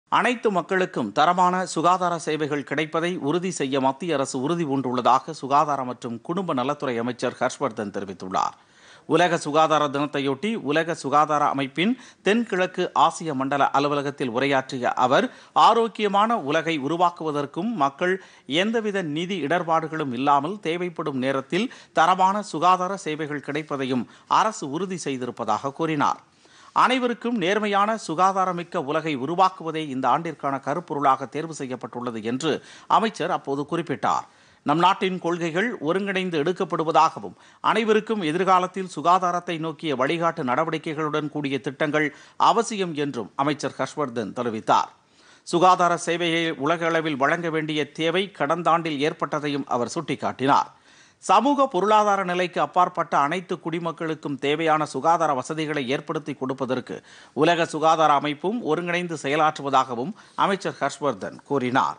அனைவருக்கும் தரமான சுகாதார சேவை
நாடுமுழுவதும் உள்ள  அனைத்து தரப்பினருக்கும் தரமான சுகாதார சேவை வழங்க மத்திய அரசு உறுதிபூண்டுள்ளது - அமைச்சர் ஹர்ஷ்வர்தன்.